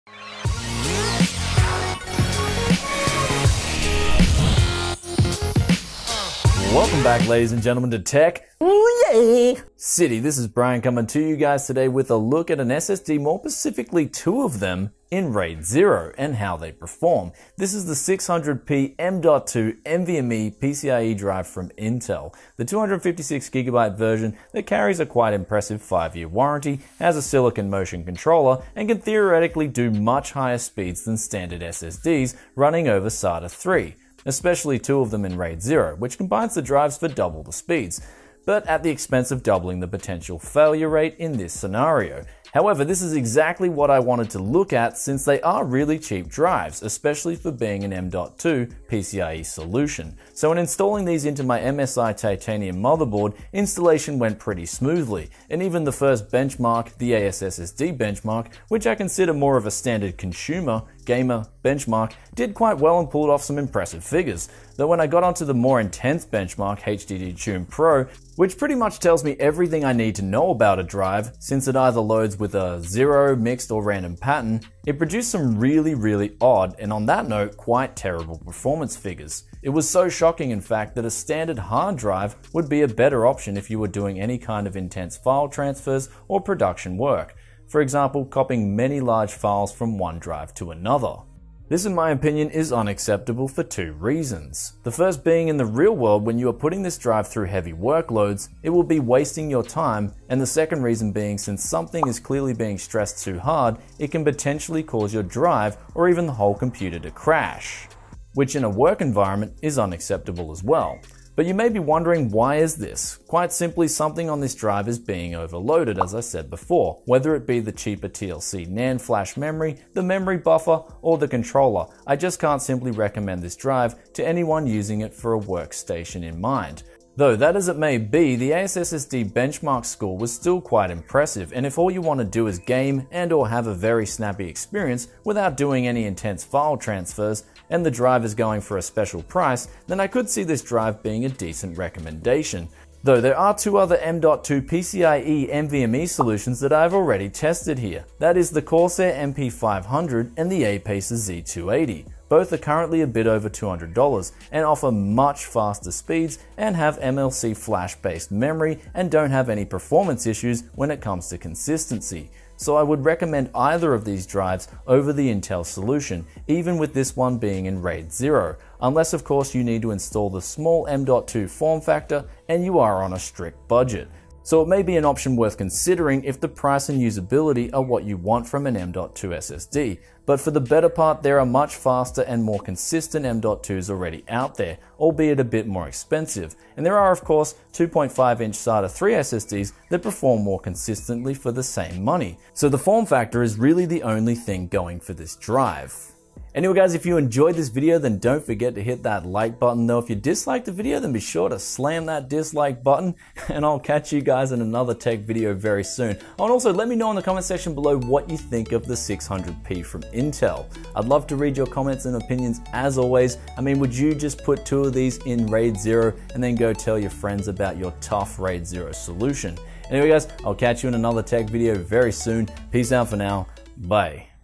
Raid 0 With The Intel 600P NVMe m.2 PCie Drives - Is it Worth it?
Today is an odd video where I was sent 2 Intel 600p drives from MSI (? maybe to showcase the awesome built in native nvme support on the titanium?) to take a look at, and everything was honestly running pretty well, it was refreshing to get back on a raid 0 setup, and honestly the benefits were looking pretty solid... until I hit the HDTune Pro benchmark where the performance starting literally shitting itself to the likes that I have never seen before with a drive (even worse than the BX100 that I had in the past).  Though the 5 year warranty is pretty solid, and if all you are doing is gaming and get pick this thing up for a real bargain (much cheaper than retail)